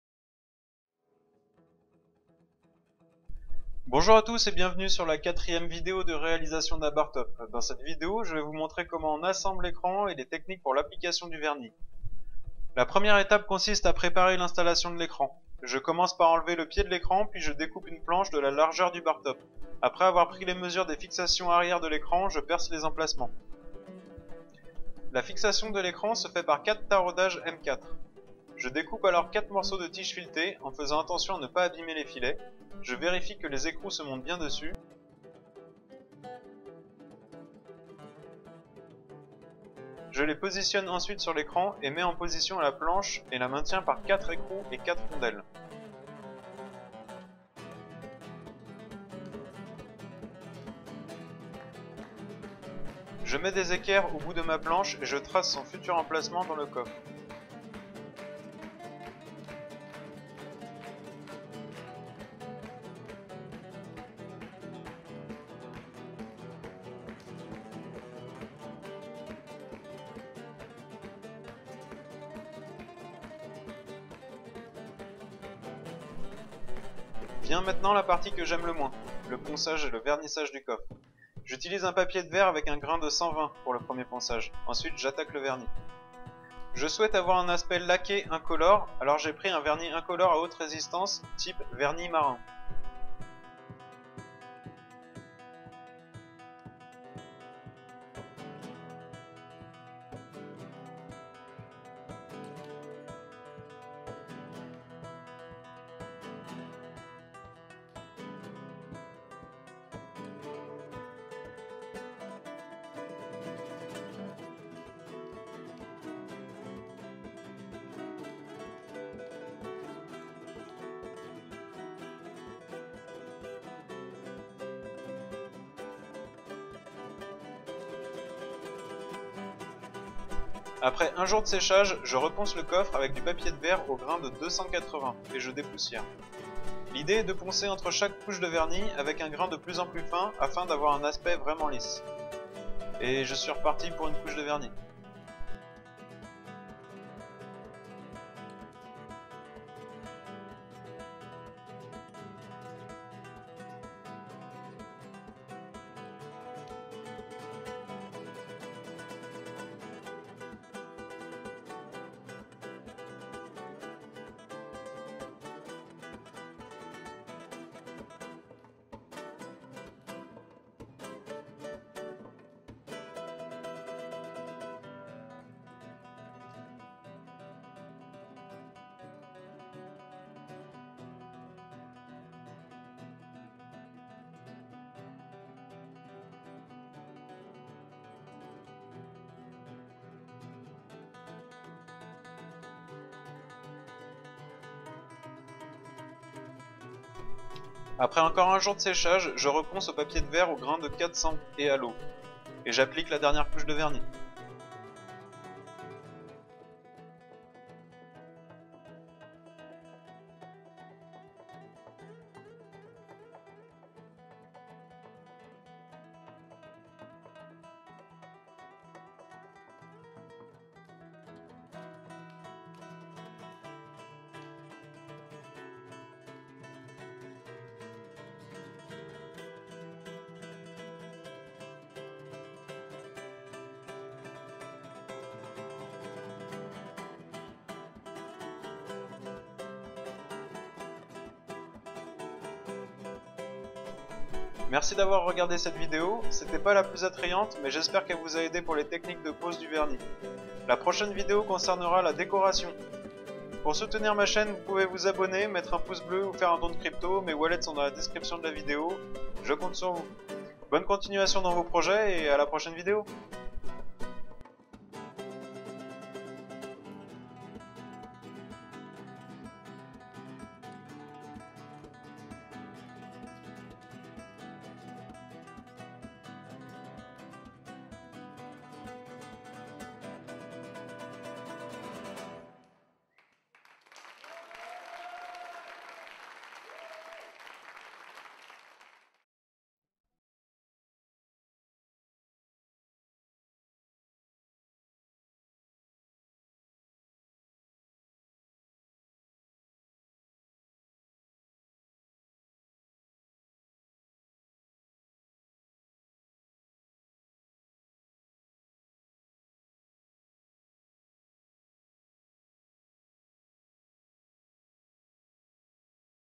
Bartop #4 - Esthétique coffre
Le matériel :
- Bois sapin/pin 2000x60x18 - magasin de bricolage
- Planche supplémentaire - magasin de bricolage
- Plexiglas 1000x500x2
- Visseries Ø3x13 Ø4x15 Ø4x20 etc.
- Charnière piano 1m
- Multiprise 5 port
- Adhésif noir
- Rallonge USB se montant sur un panneau
- Grenouillère de fermeture capot arriere
- Grilles pour enceintes
- Patins caoutchouc antiglisse
- Boutons blanc et noir, joystick avec controleur USB
- Papier de verre
- Vernis bois haute résistance (marin)
- Aimants 8kg
- Insert m6
- Poignées de transport
- Câble HDMI DVI
- Kit Raspberry alim carte 16Go boitier
- Carte SD 64Go
- Bande LED RGB 5050 reagissant a la musique
- Ampli audio
- 2 enceintes 10w
- Alimentation amplificateur
- Câble rouge et noir
- Lot de stickers
- Ecran 20"

Pour faire un don en crypto monnaie (Réception wallets) :
BTC : 148wZrieLKn2CpNiE3rnLf5ECs28JDKcEK
BCH : qqf843ewz8h9rmhcfhuzt0za837397kfxqdxuded7n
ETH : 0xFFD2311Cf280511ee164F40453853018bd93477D
LTC : LKjfZ8AuzDdYuFPfHbnPZ81FQG1egfibL3